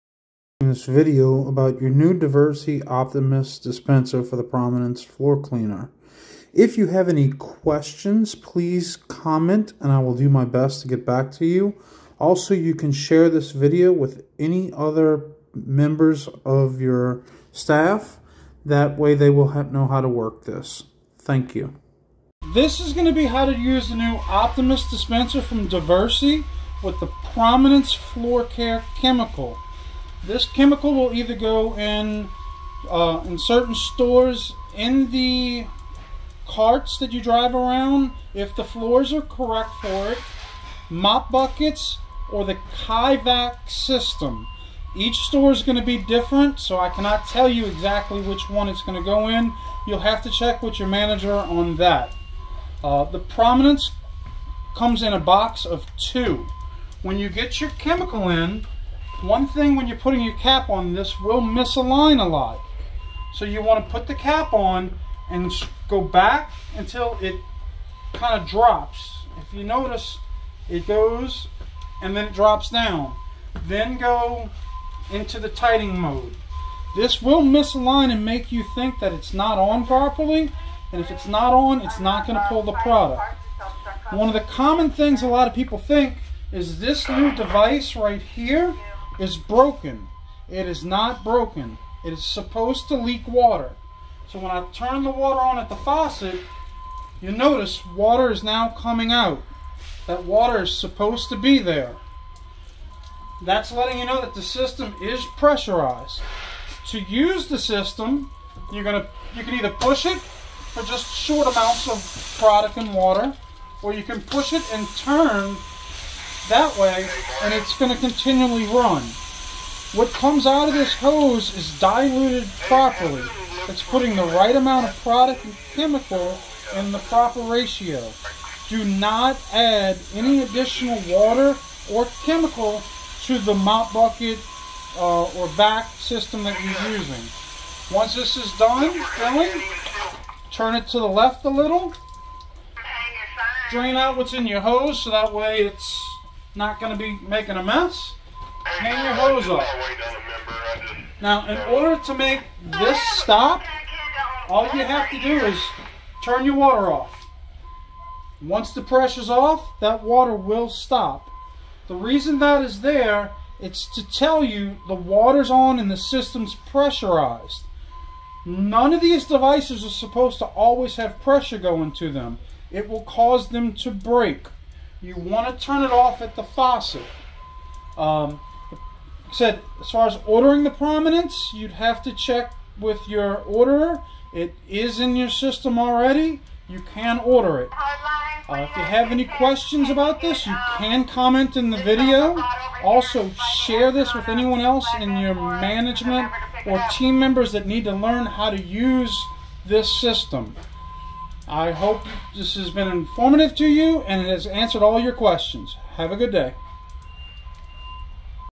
Diversey Sam's Club - How to use Optimus dispenser for Prominence floor care chemical
Please watch the entire video. Share with other associates that might need to learn how to use this device with the new Diversey floor care chemical. Any questions you might have, you can ask them in the comments and I will reply. Make sure to read all comments to see if your question has been asked and/or answered already.